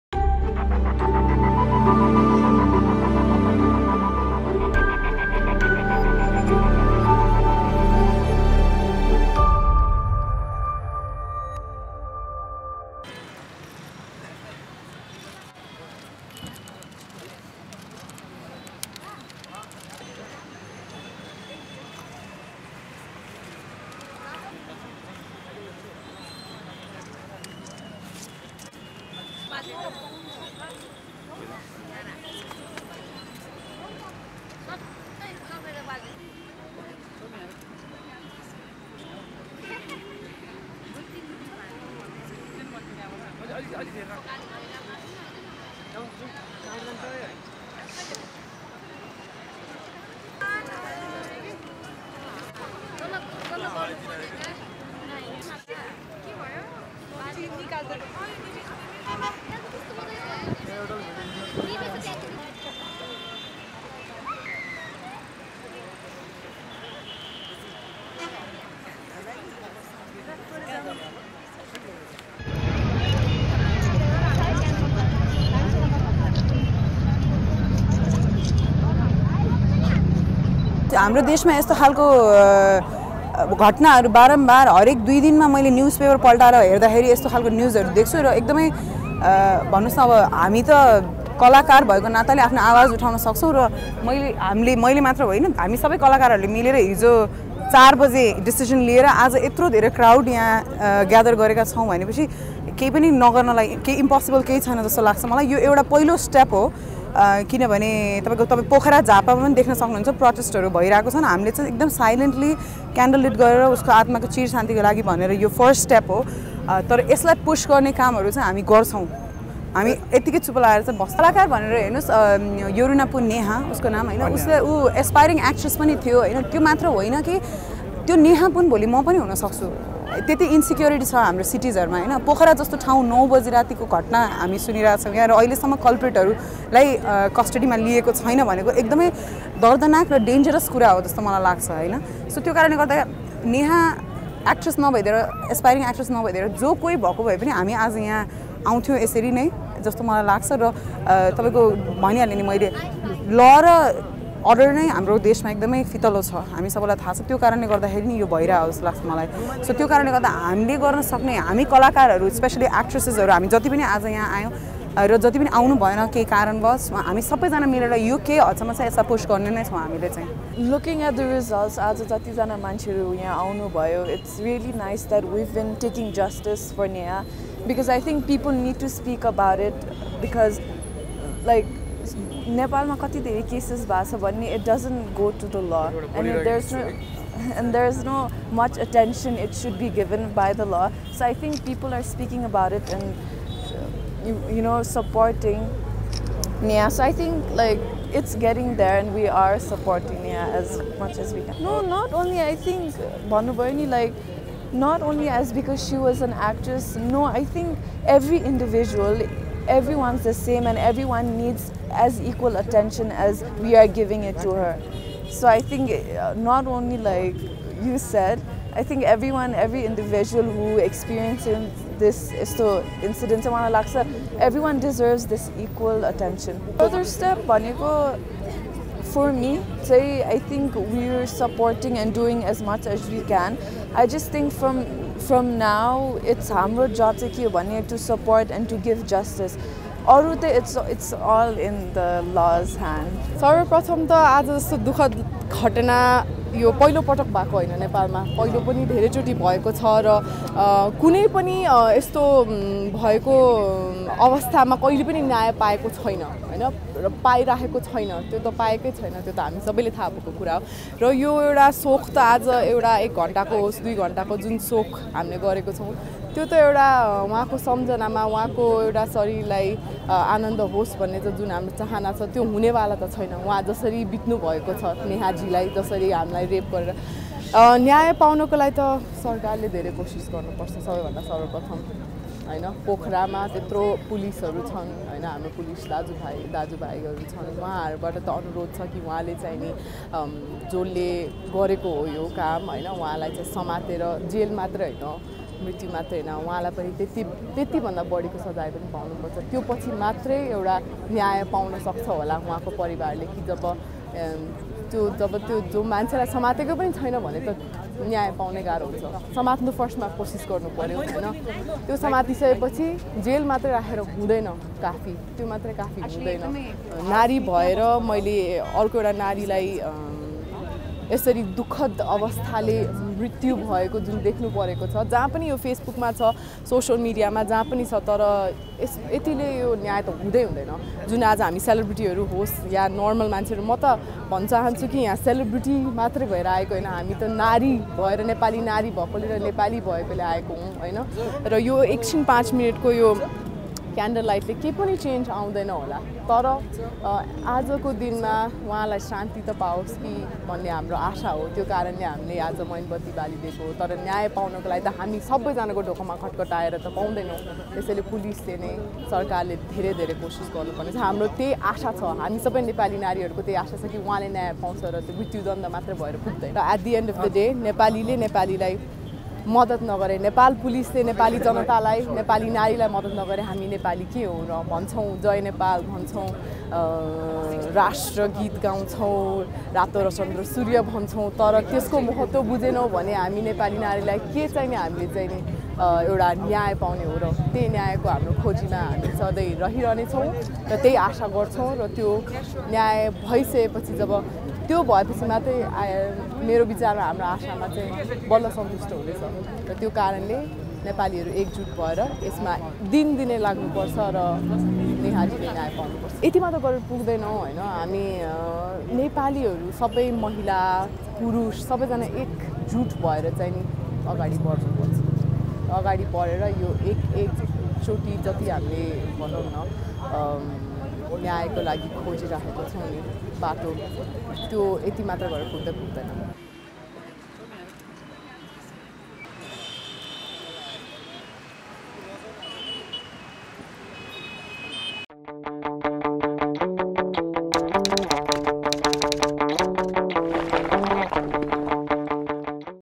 Justice For Neha Pun | Richa Sharma Miss Nepal Evana Manandhar at Maitighar Mandala
view more